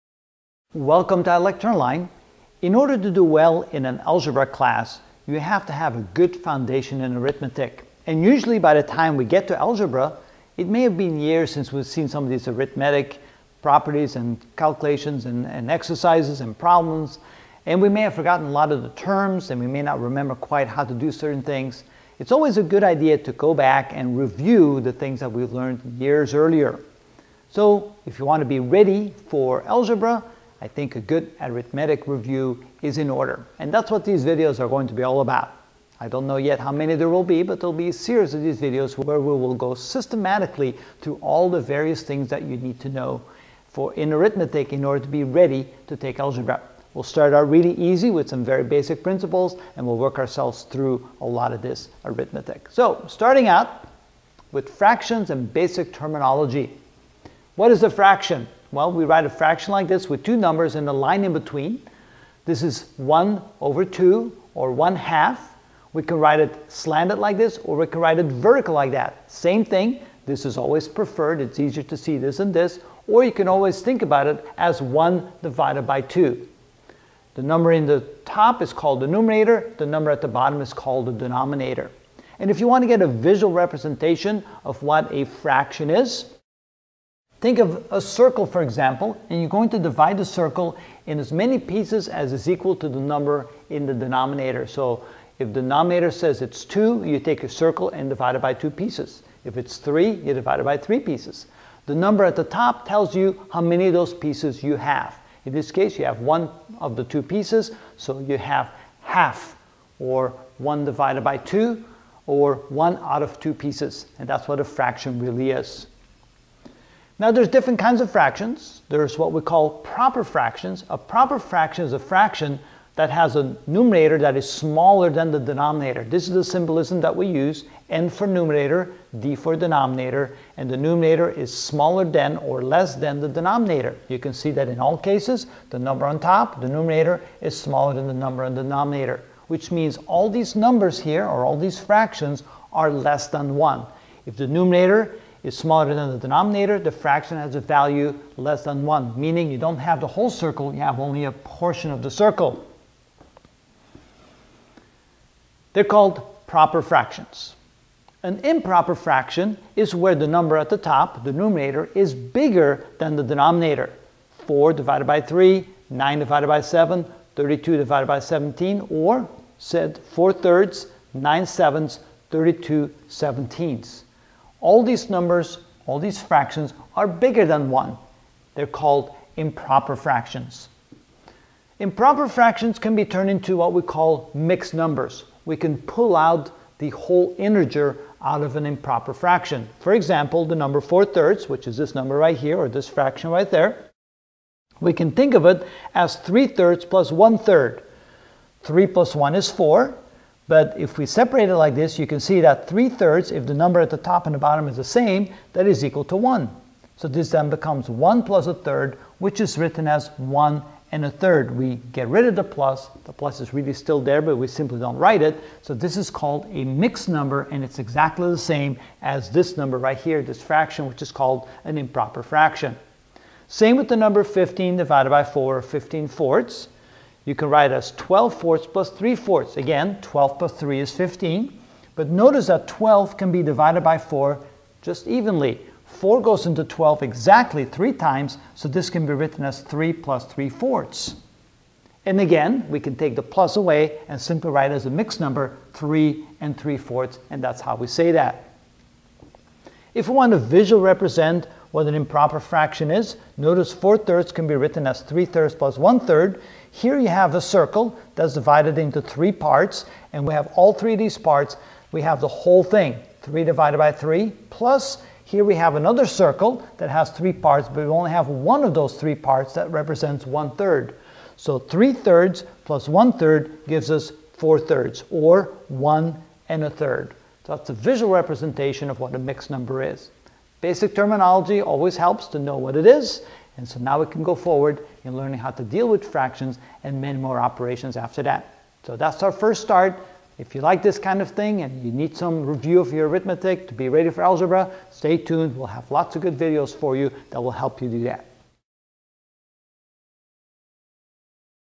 Math: What Every 5th Grader Should Know: Algebra Prep (1 of 58) Ch 0: Fractions - Basic Terminology
In this video I will explain the basic terminology of fractions: numerator, denominator, proper and improper fractions, and mixed numbers.

Next video in this series can be seen at: